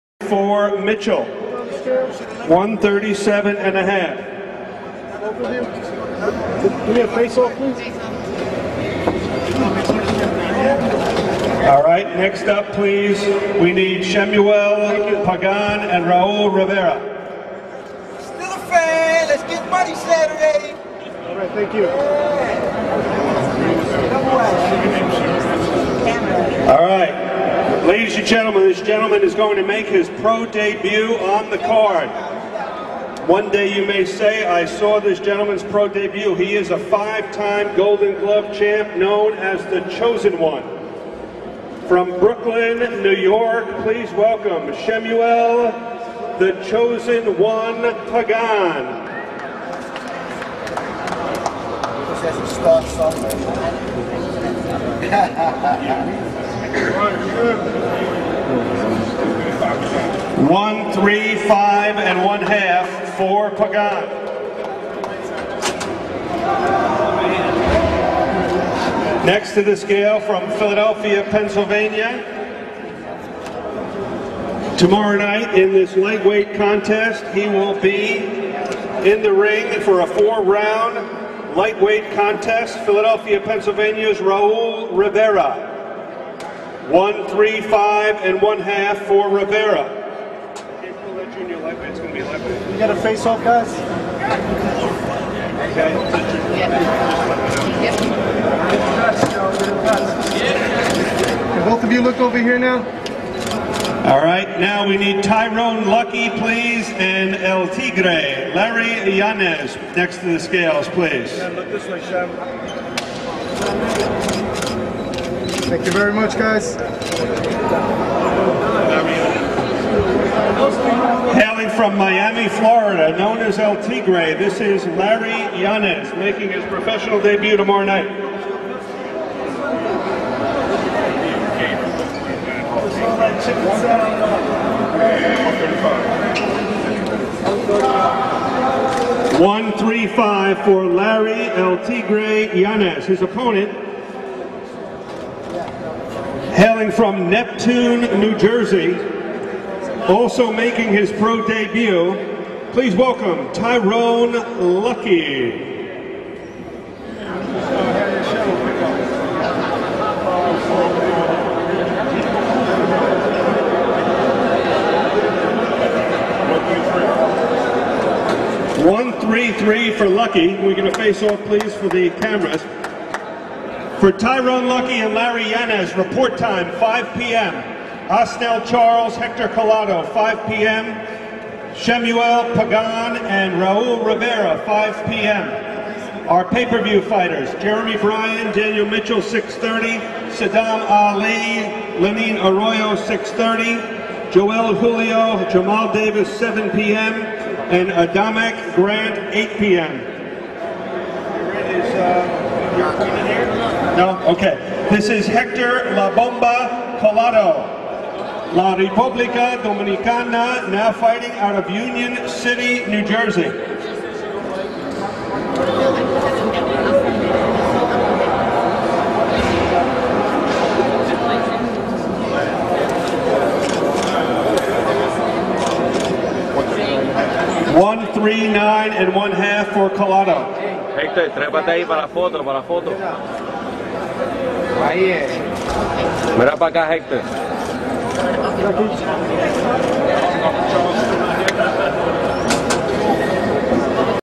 TOMASZ ADAMEK VS MICHAEL GRANT WEIGH IN PT 2
Tomasz Adamek vs Micheal Grant weigh-in at the Prudential Center in Newark, NJ. The fight takes place this Saturday August 21. They will be fight for IBF International Heavyweight and NABO Heavyweight Championship.
Video By Bradley Orengo.